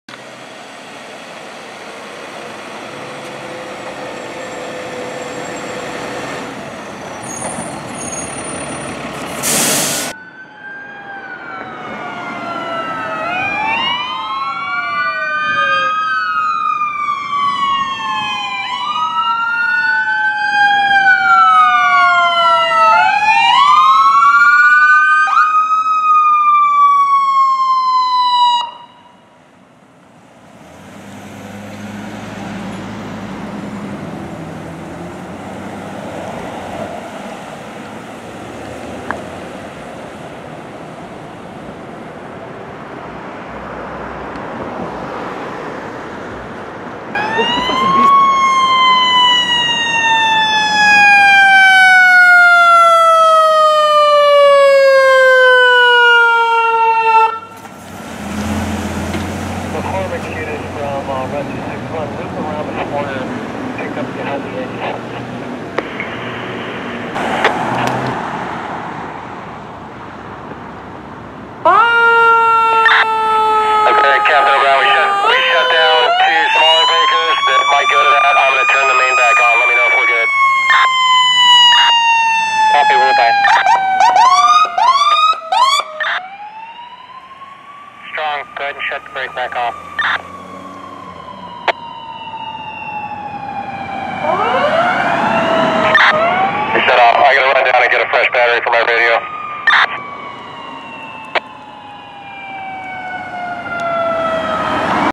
Redondo Beach Fire Dept. Battalion 62, Engine 62, Rescue 61
Redondo Beach Fire Dept. Engine 62, Rescue 62, Battalion 62 (reserve for Battalion 61), & McCormick 1001 arriving on scene of an unconscious person in a bathtub on Sepulveda Blvd. & Rosecrans Ave. in the City of Manhattan Beach.

Units due: Engine, Rescue, Battalion 62, McCormick 1001.

Redondo Beach Firefighters provided coverage over the City if Manhattan Beach while they were tied up on a commercial structure fire.

Manhattan Beach, CA - May 10th, 2018